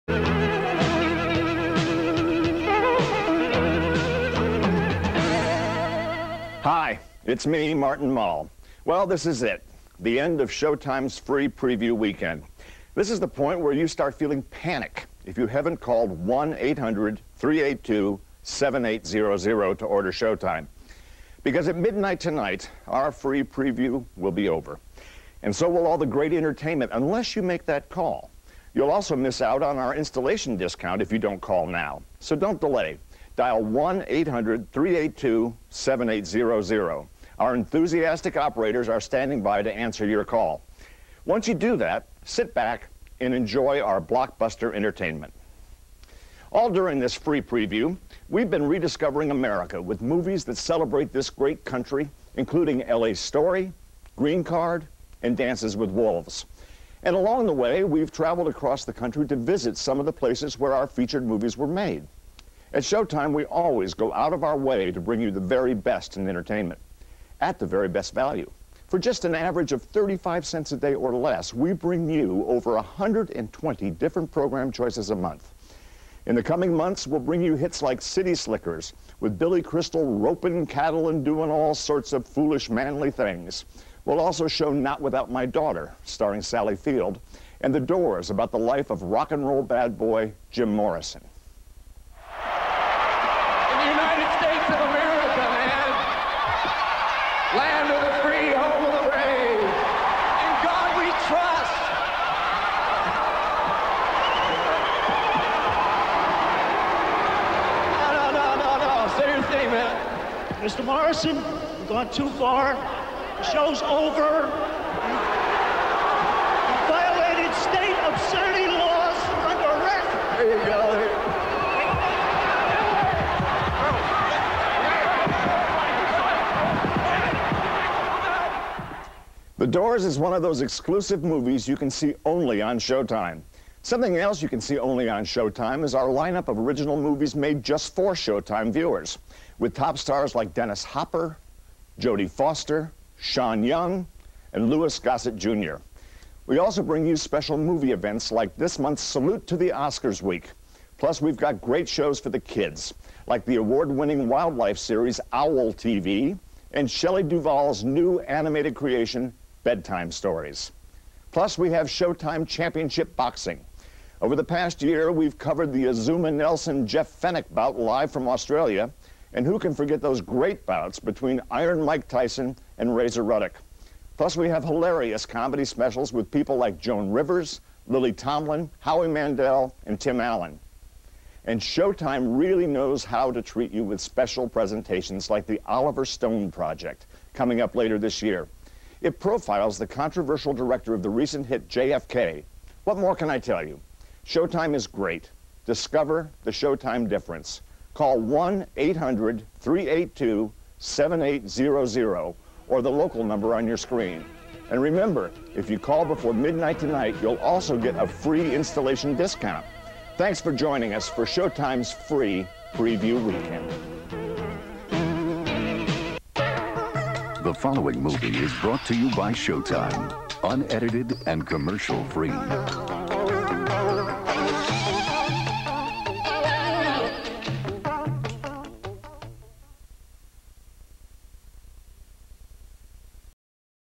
Showtime Free Preview Bumper (circa 1991)
Not 100 percent sure about the year. Found it on a tape that had "Dances With Wolves" on it, so it must be from the early 90s, late 1990 or 1991.